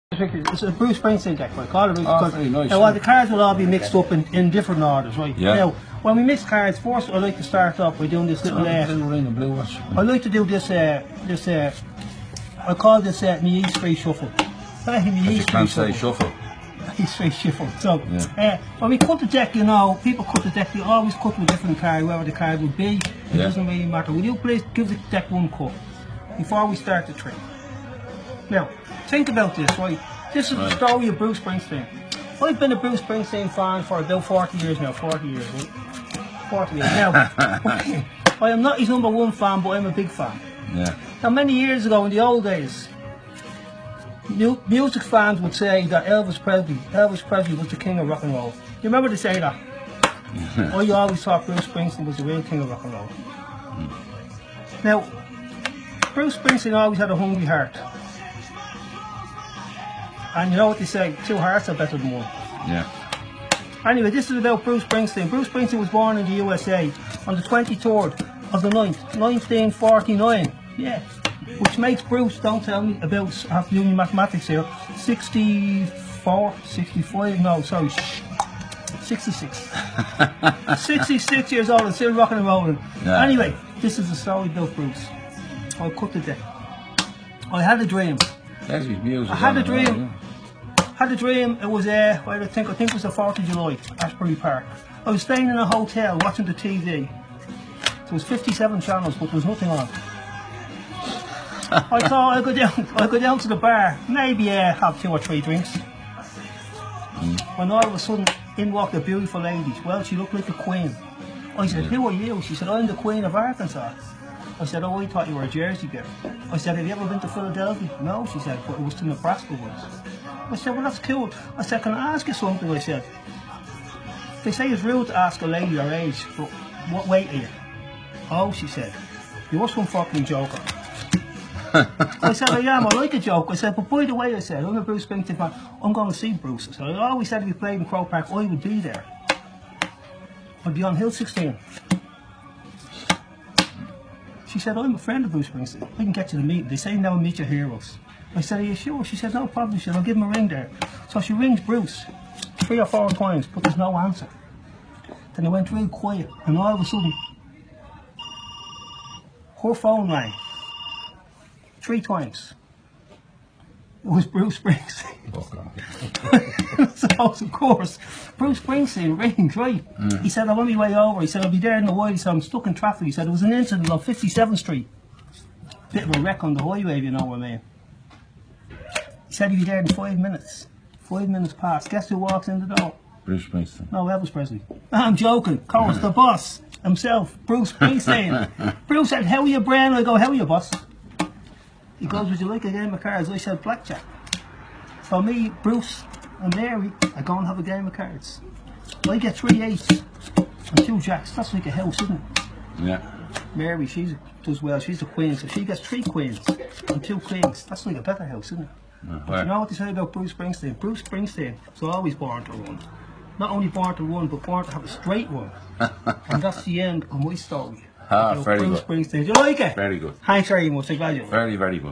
LiveLeak - Amazing Card Trick Inspired by Bruce Springsteen
Doing a card trick and telling a funny story are both difficult things to do, in this video, Brendan Harte manages to combine both into a remarkable tale about Bruce Springsteen that uses every card in the deck, plus a few extras that somehow slipped in. Credit: Facebook/anthony.coogan.9